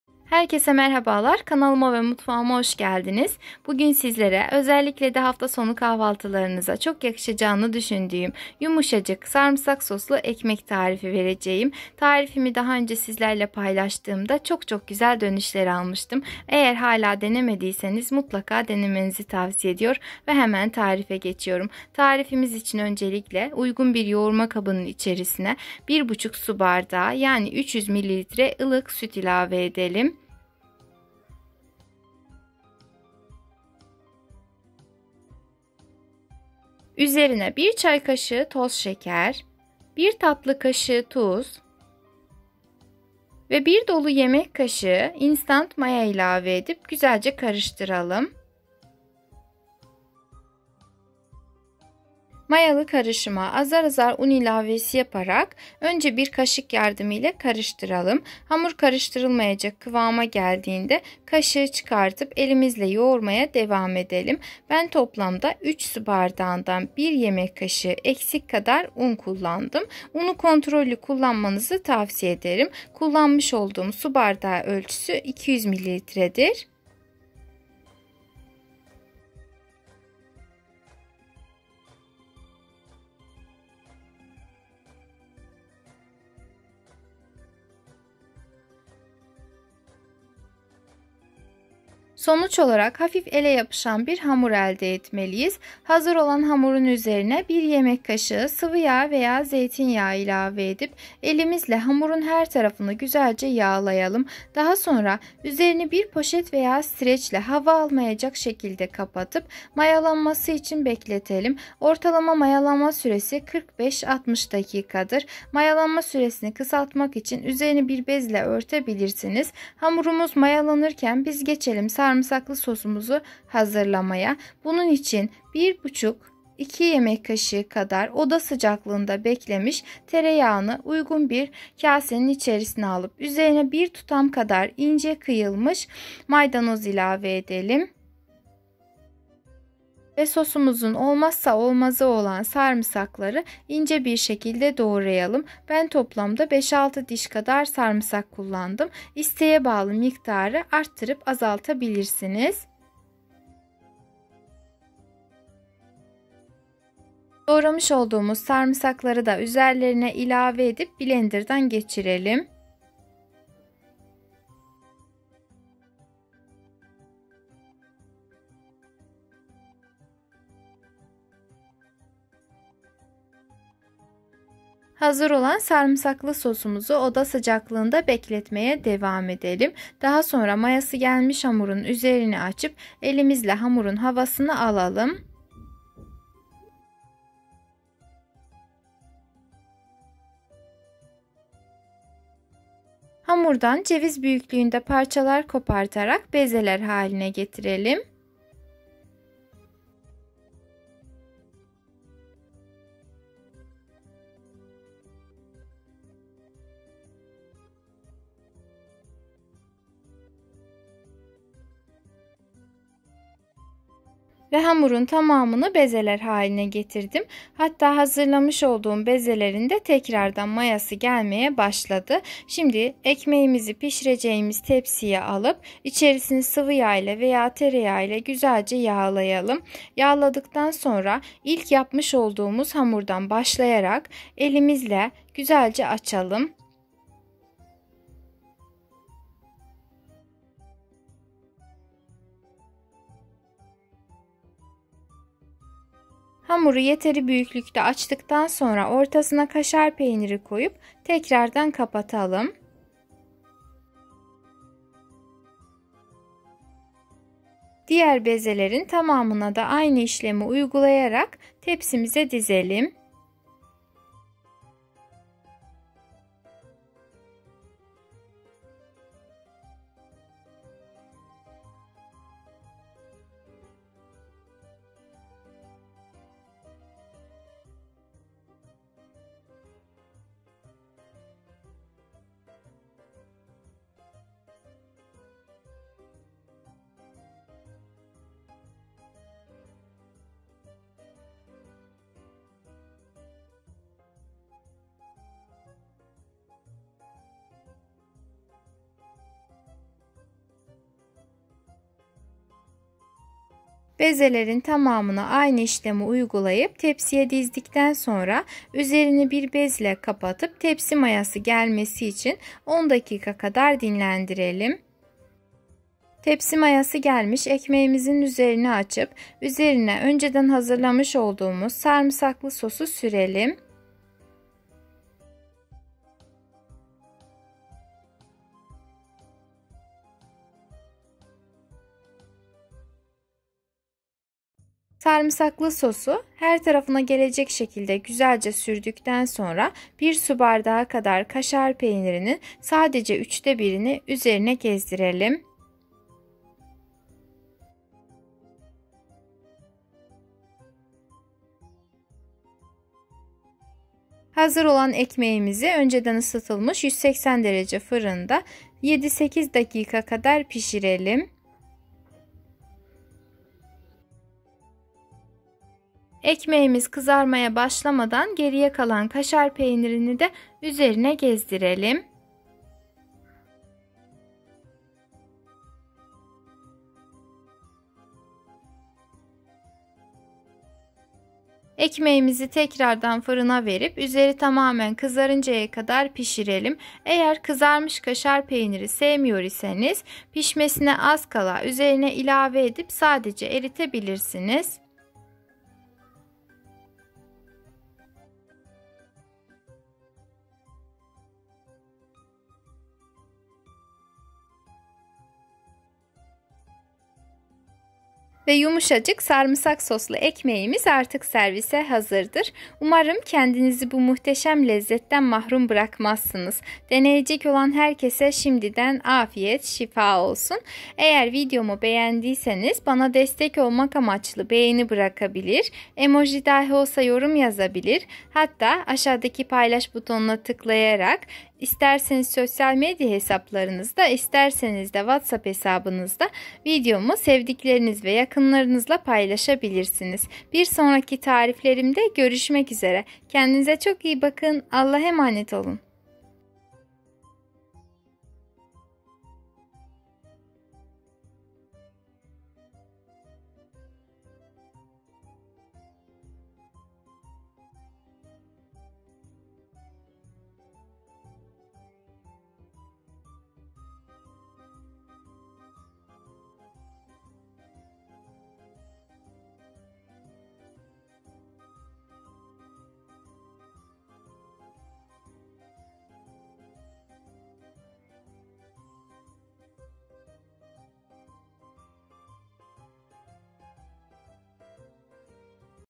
10 Bin kişinin Defterine Eklediği👏 Deneyenlerin Vazgeçemediği Sarmısaklı Ekmek Tarifim /Garlic bread
10 Bin kişinin Defterine Eklediği👏 Deneyenlerin Vazgeçemediği Sarmısaklı Ekmek Tarifim /Garlic bread

 Bu videomda Deneme rekorları kıran Sarmısaklı ekmek tarifimi daha farklı  bir şekil vererek  sizlerle paylaştım. Bu haliyle çok daha yumuşacık ve lezzetli oldu🤭 Deneyenlerin 99%  tarifi tekrar denediği  hatta vazgeçilmezi olduğunu  söylüyor. Eğer hala denemediyseniz mutlaka denemelisiniz ☺

Malzemeler:
300 ml ılık süt (1,5 su bardağı)
5 gram şeker(1 çay kaşığı)
10 gram tuz(1 tatlı kaşığı)
340 gram un(3 su bardağından 1 yemek kaşığı eksik)
10 gram zeytinyağı(1 yemek kaşığı)
10 gram instant maya
(1 dolu yemek kaşığı)
sos için:
5-6 dış Sarmısak
1 tutam maydanoz
50 gram tereyağı (2 yemek kaşığı göz kararı)

üzeri ve arası için:
kaşar peyniri